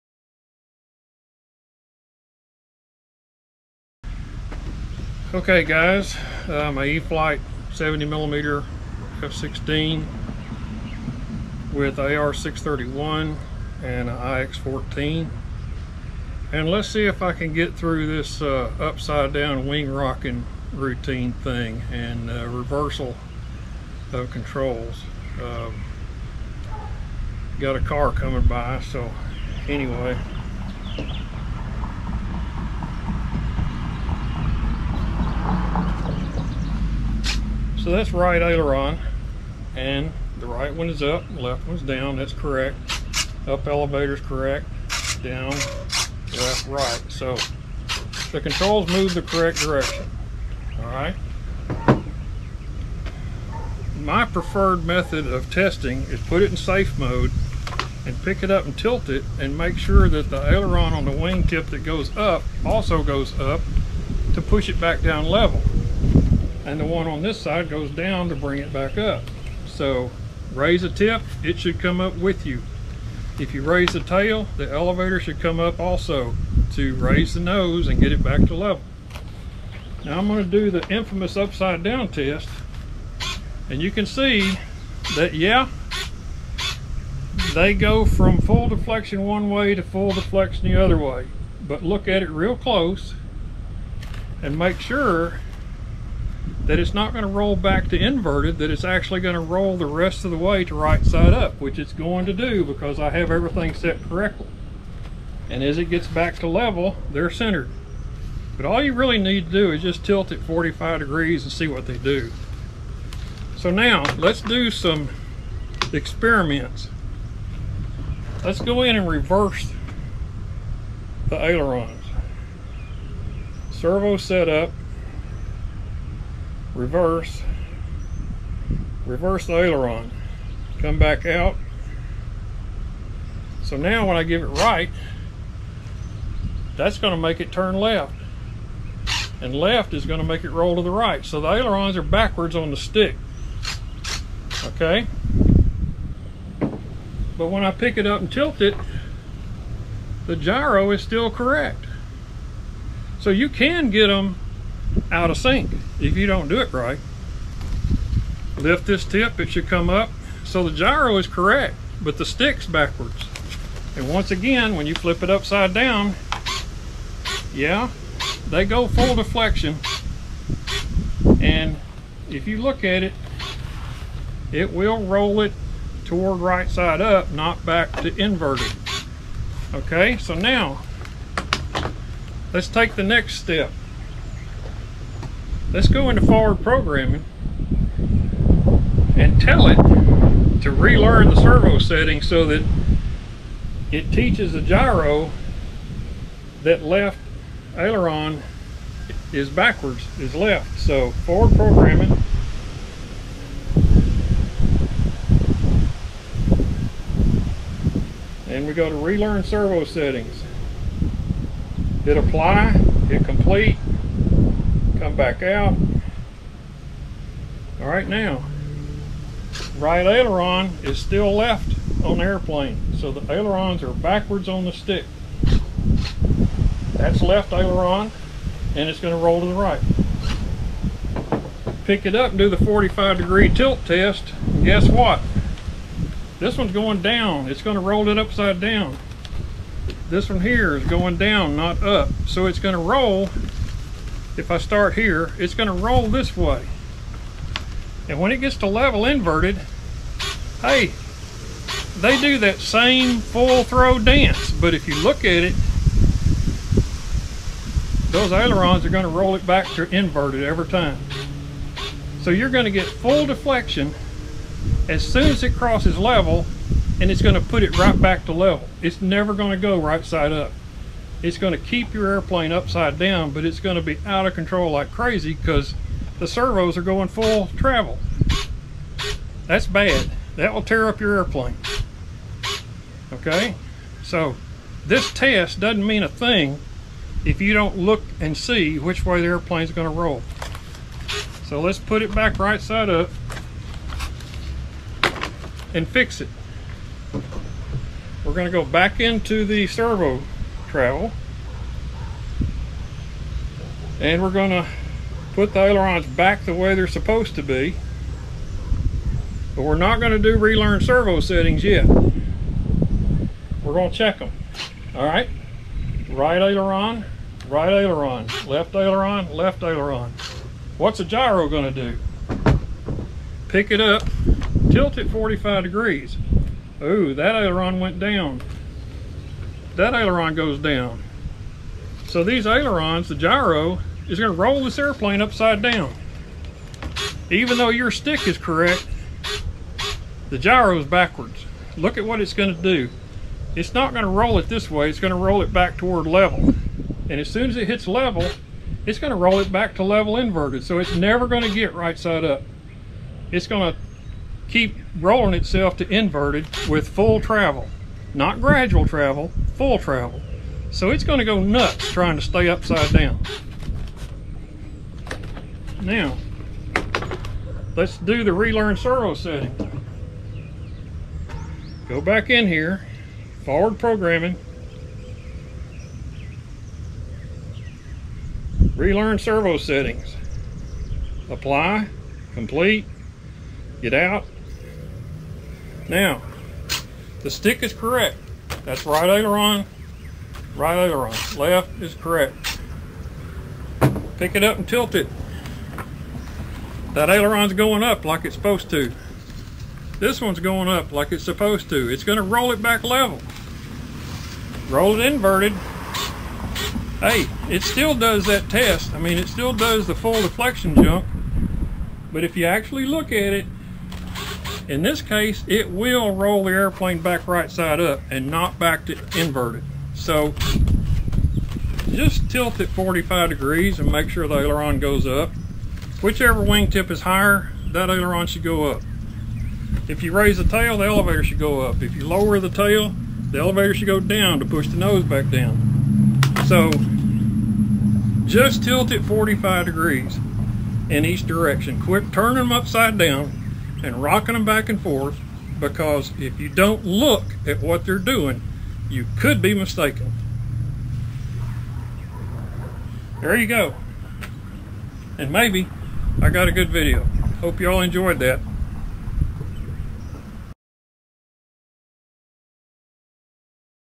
Spektrum AS3X/SAFE Gyro Testing - 6 May 2023
Stop doing the inverted wing-rocking test! Just because the ailerons zip back and forth doesn’t mean they’ll roll your plane back to right side up. You have to look at which way they deflect. All you need to do is put the gyro into SAFE mode and tilt the plane about 45° to the left, right, nose up, and nose down and make sure the control surfaces will make the plane go back to level right-side-up flight. You can also do this check in AS3X mode, but the surfaces will only deflect while you're moving the plane, so it's a little harder to tell what they're doing. Also, you must open the throttle to at least 25% to activate AS3X and then bring it back to idle.